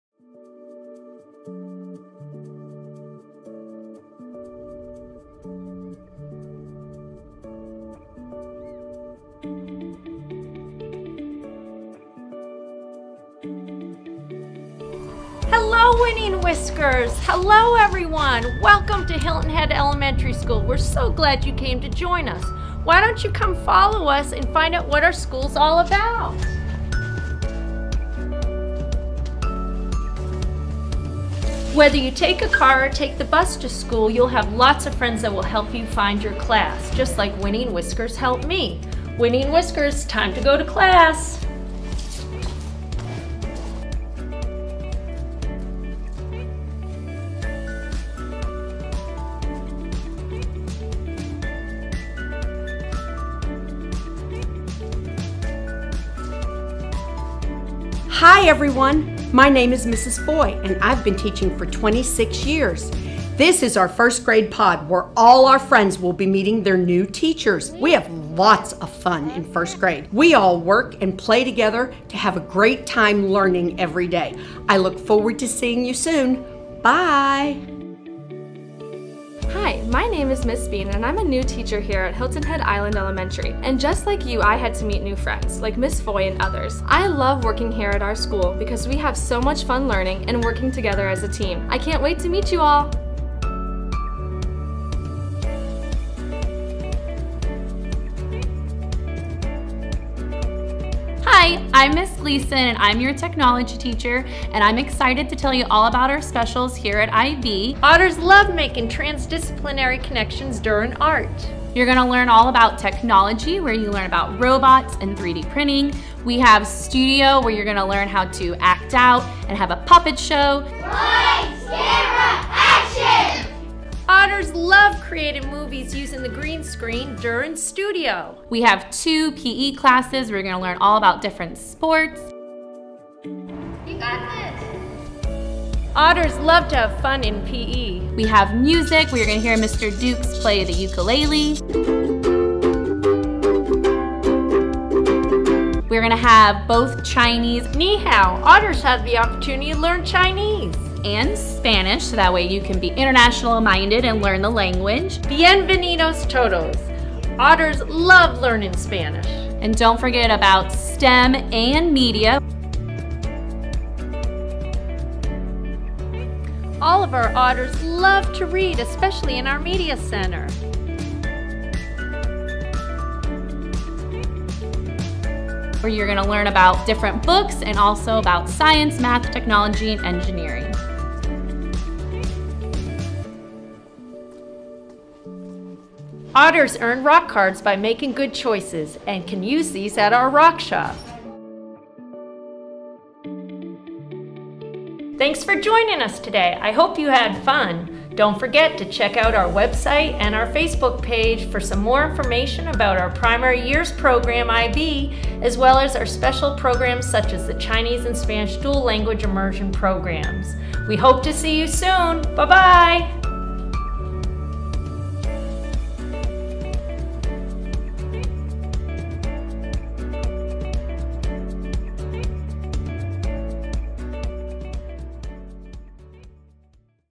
Virtual Tour - Hilton Head Island Elementary School
The new school year will be here soon and Hilton Head Island Elementary School wants to invite all of our soon to be first grade students to take a tour of their new school.